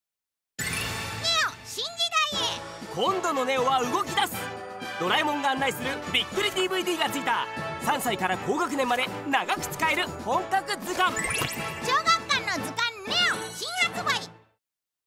図鑑NEO テレビCM
今度のNEOは動き出す！とのび太がナビゲートする、驚きの自然映像満載のDVDがついている！！
3歳から高学年まで長く使える本格図鑑。
詳しくはHPをご覧ください。